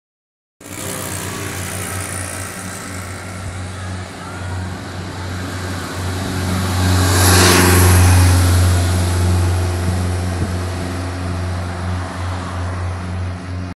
Breng Berkhof Premier AT 18 op hulp motor
Vanwege verkeershinder rijden de trolleybussen bij Arnhem cs op hulp motor